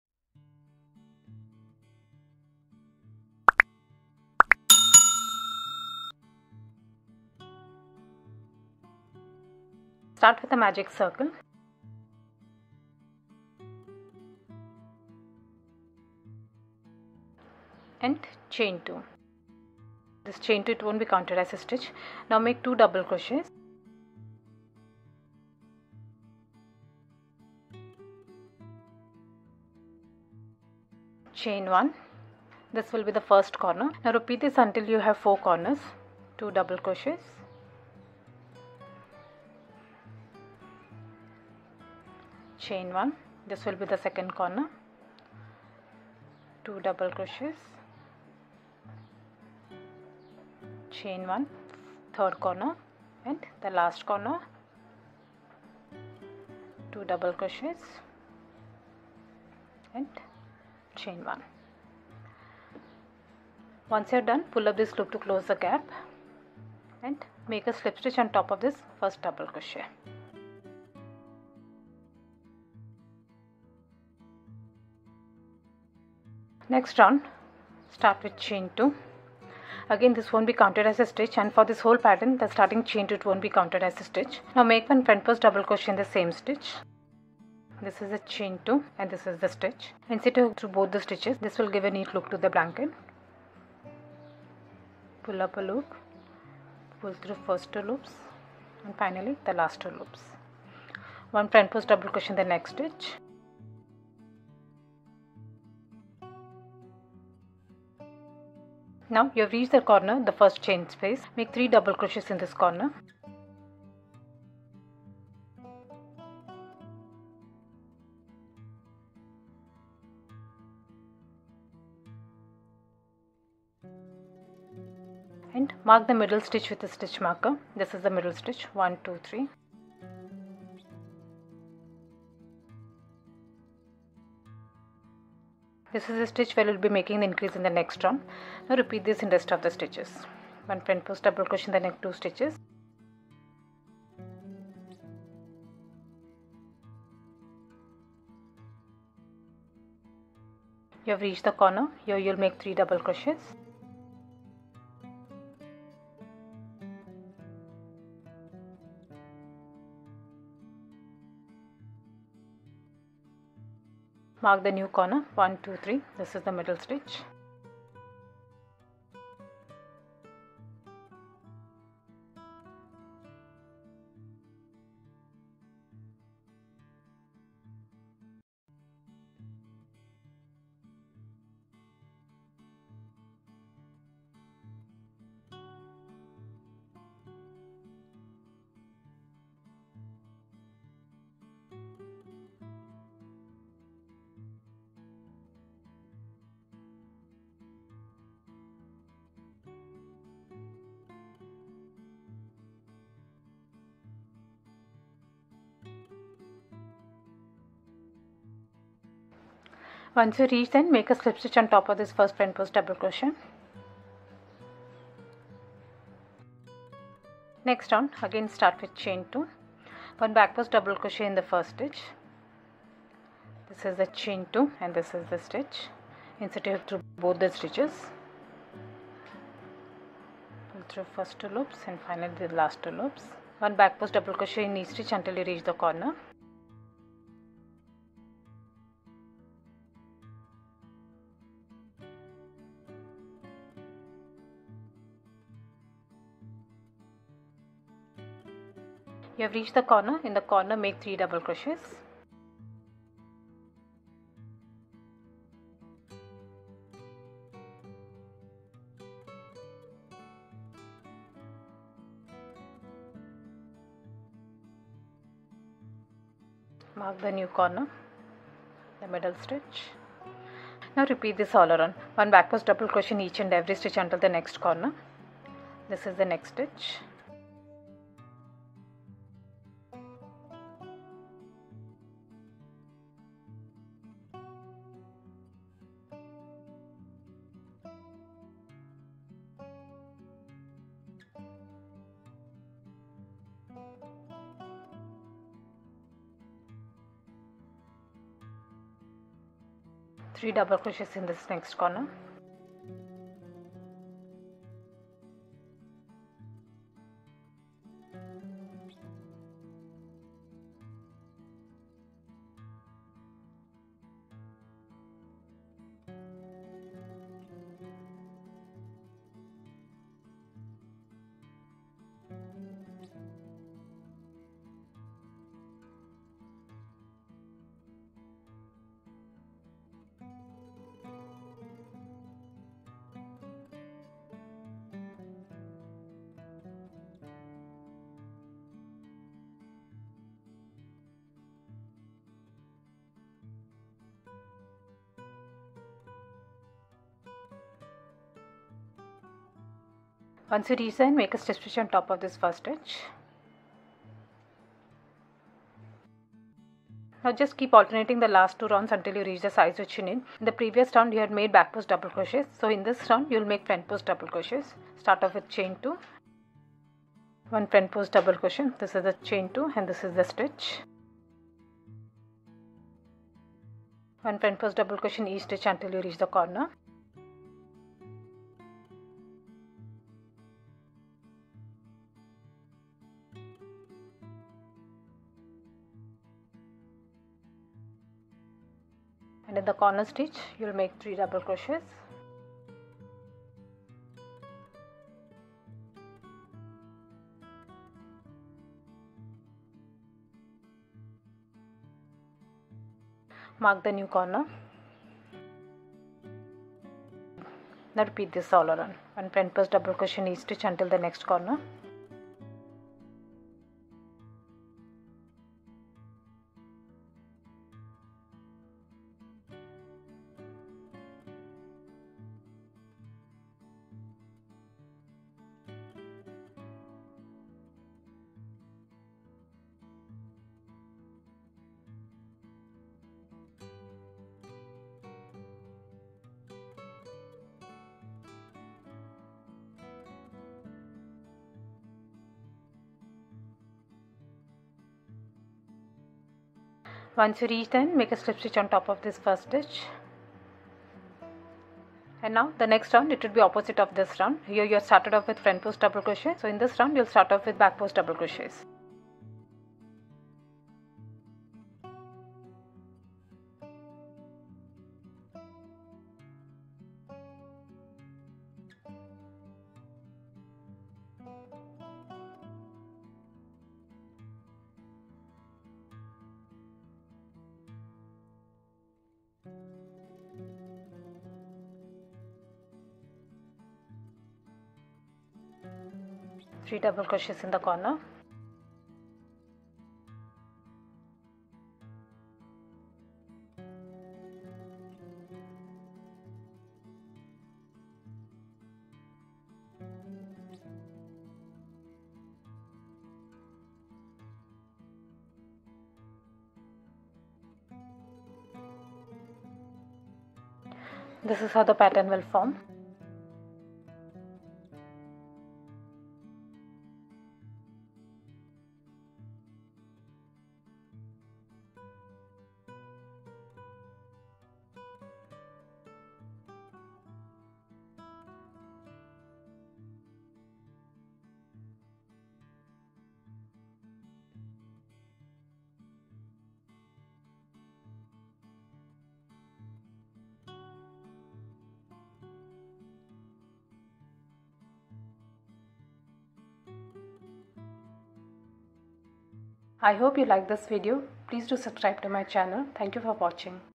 Raised Square Crochet Blanket Motif Easy Crochet Afghan Square Tutorial
Crochet Square Blocks or Crochet Afghan Blocks and Crochet Motifs are the simplest and the most versatile patterns to whip up. There are many different ways to transform and tweak the crochet square. You can use these crochet squares to make a crochet blankets, pillows, crocheted bags , placemats and tablemats, crocheted dishcloths and so much more. The possibilities are just endless.

Here is the pattern for a quick and very easy Raised Square Crochet Blanket Motif. This Crochet square works up super quick and they can be made to any size as required. You can join and crochet them into any project of your choice. Join them together to make a pretty and snuggly comfort blanket or If you are not keen on making a join as you go blanket, you can also keep making the repeat rounds to make a large size crocheted blanket. Either way , Crochet Square blankets are fun to make. Join up the squares to make blankets of any size or simply use it individually as crochet coasters or crochet potholders.

This pattern is also available as an ad-free, printer-friendly pdf pattern that can be purchased on Ravelry

Materials Used:-
8 Ply Cotton Variegated Yarn
4 mm Crochet Hook

Size:-
10 inches Block- Customizable to any size

Notes:-
For this whole Pattern, the Starting Chain 2 is not counted as a stitch. It is Just to bring the yarn to a working height. Start the next stitch in the same stitch.

Time Stamps:- Select the time to jump directly to any Specific Round
0:00   Intro
0:10   Round 1
1:16   Round 2
3:37   Round 3
5:57   Round 4 Onwards